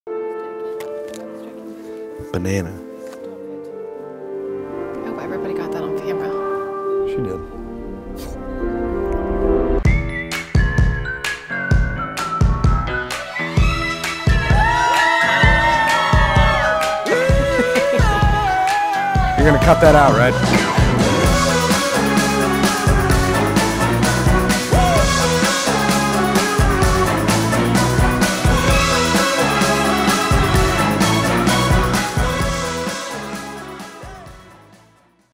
Groom Says Hilarious Things During Wedding | Sarah & Shaun Sneak Peek Video
Sarah and Shaun were married in Ohio and had everyone laughing the day away. Watch this short sneak peek video for a glimpse of their fun wedding!